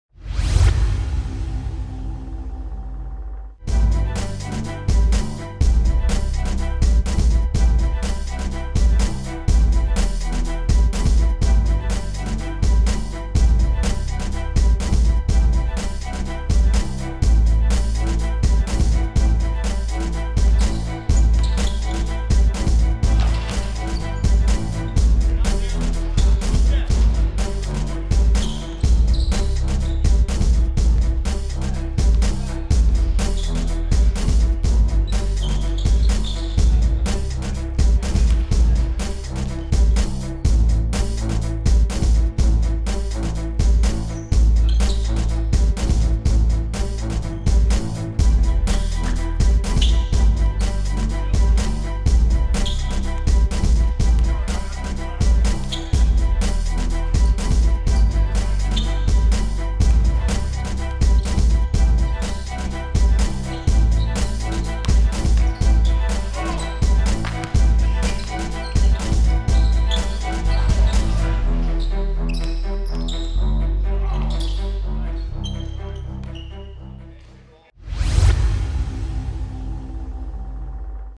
Mamadou Ndiaye 2016 NBA Pre-Draft Workout Video (Condensed Version)
U.C. Irvine big man Mamadou Ndiaye works out in preparation for the 2016 NBA Draft in Los Angeles. NBA Draft scouting video. NBA prospect. DraftExpress. NCAA basketball.